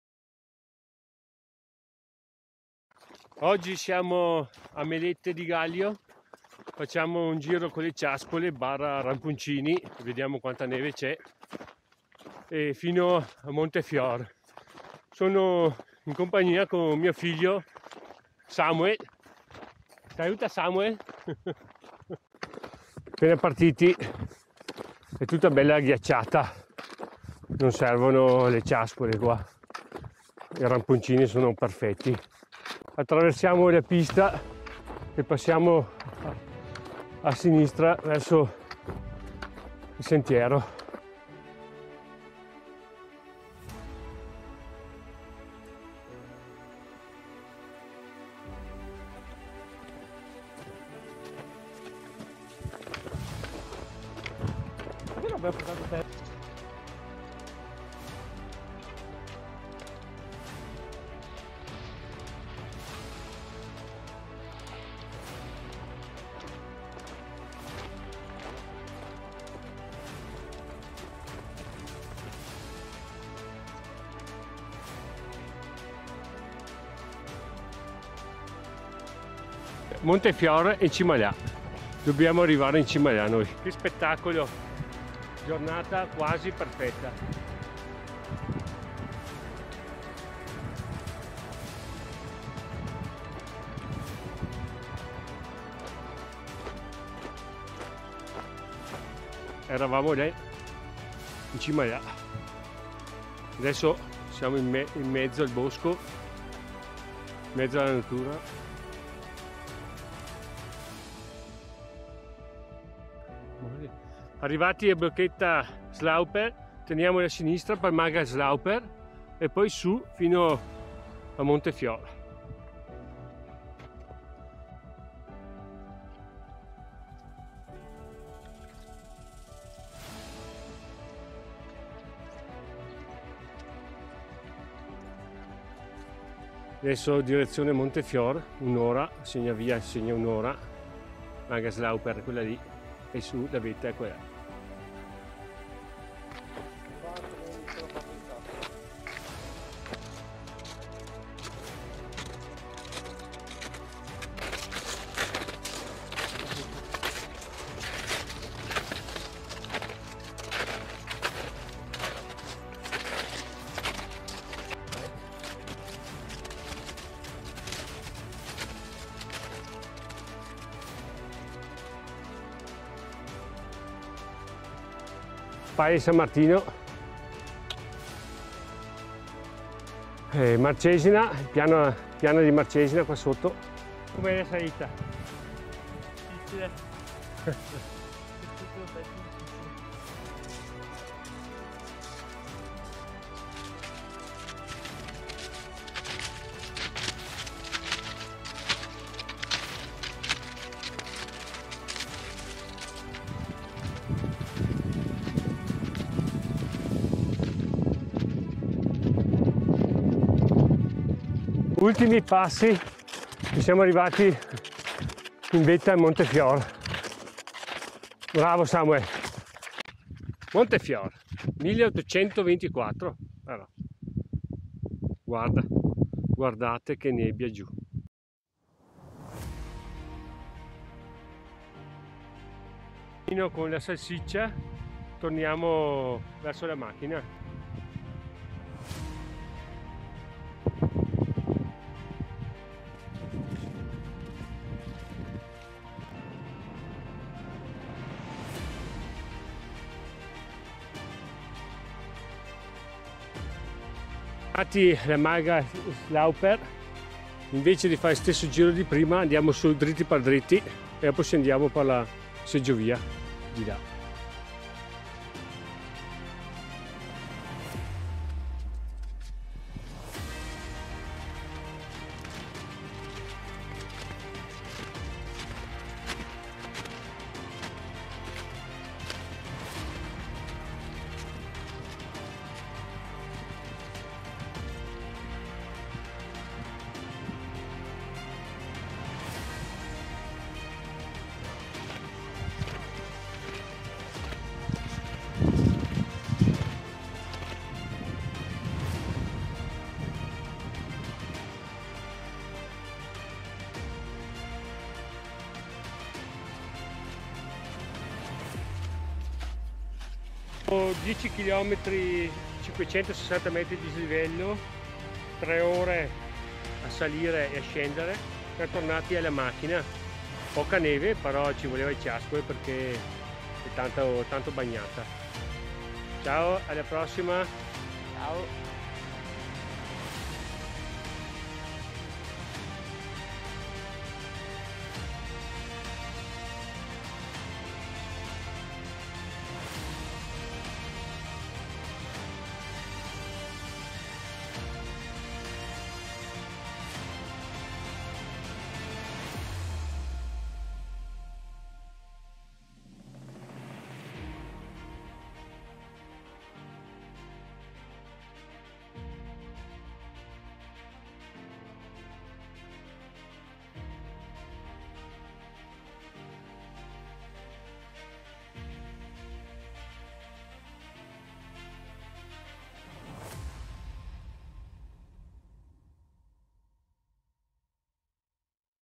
Panin onto sul monte Fior con le ciaspole.Varda dove che te porto.
Salita al monte Fior,partendo dalle Melette di Gallio.